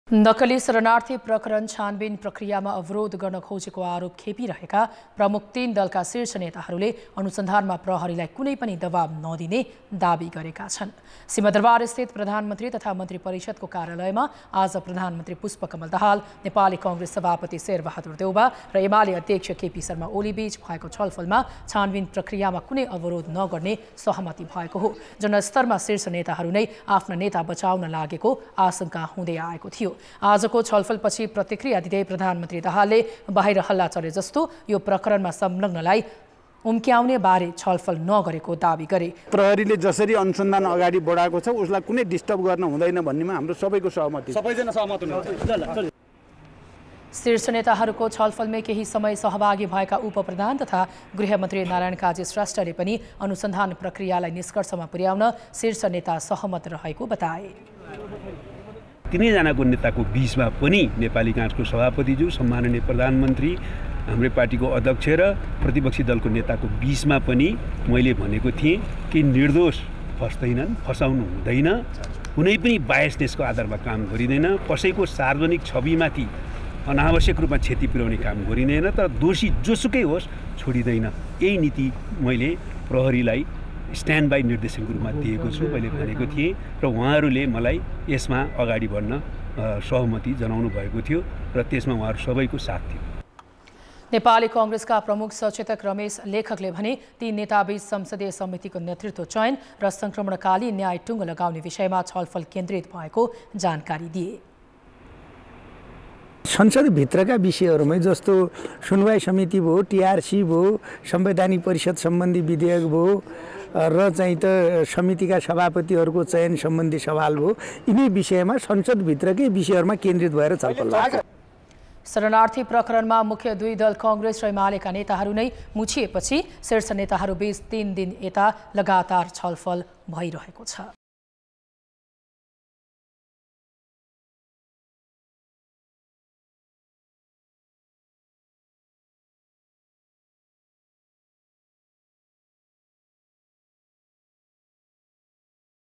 शरणार्थी प्रकरणको अनुसन्धानमा दबाब नदिने प्रमुख तीन दलका शीर्ष नेताको दाबी | Kantipur Samachar
नक्कली शरणार्थी प्रकरण छानबिन प्रक्रियामा अवरोध गर्न खोजेको आरोप खेपिरहेका प्रमुख तीन दलका शीर्ष नेताहरुले अनुसन्धानमा प्रहरीलाई कुनै पनि दबाब नदिने दाबी गरेका छन् ।